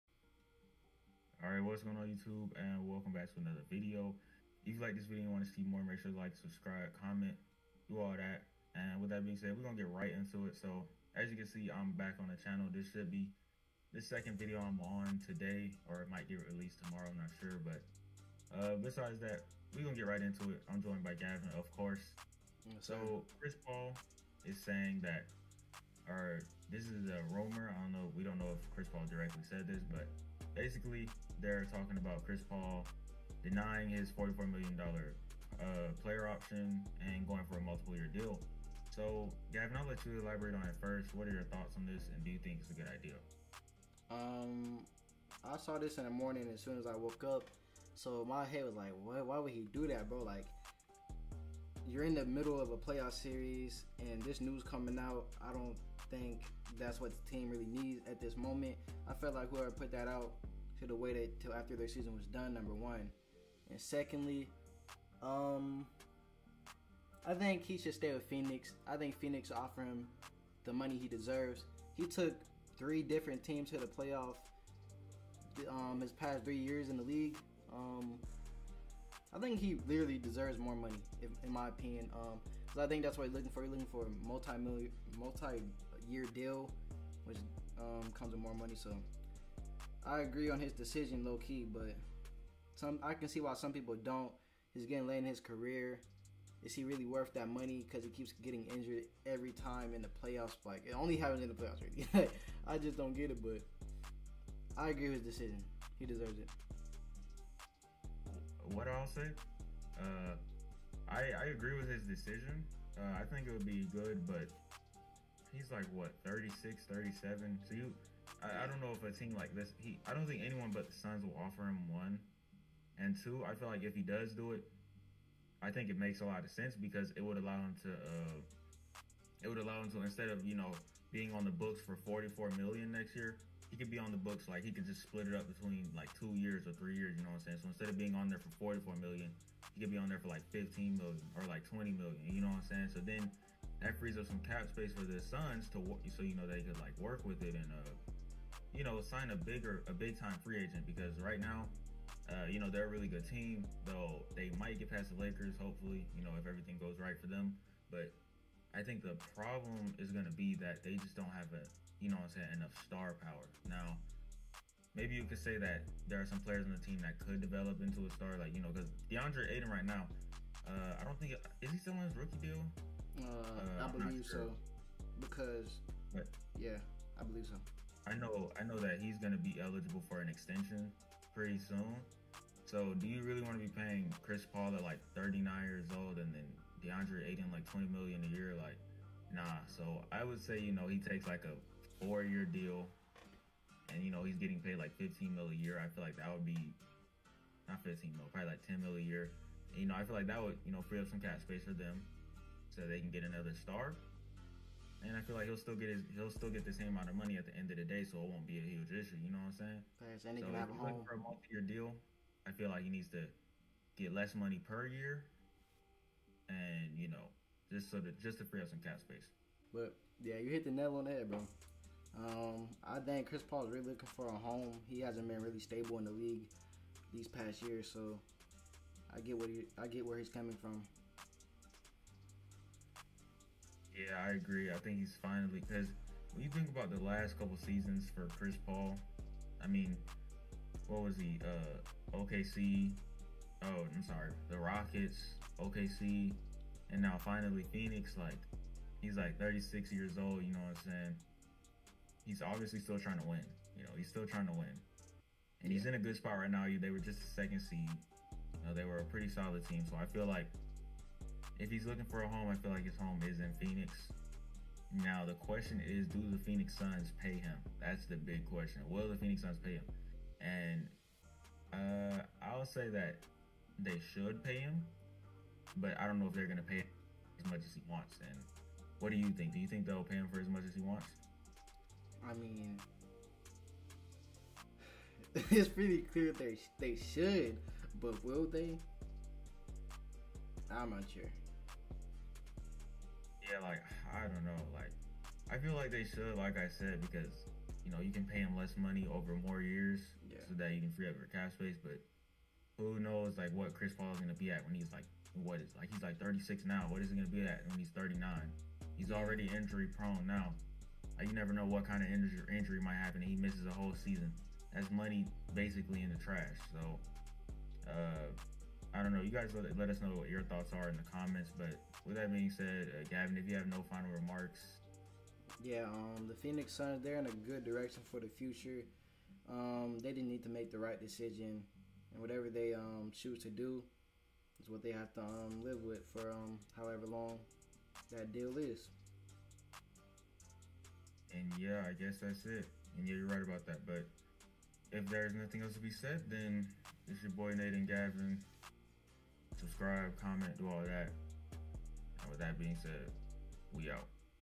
Should Chris Paul Accept His Extension? (NBA 2021)
In this video we discuss is Chris Paul should accept his 44 million dollar extension.

AG FAM ON TOP only time will tell.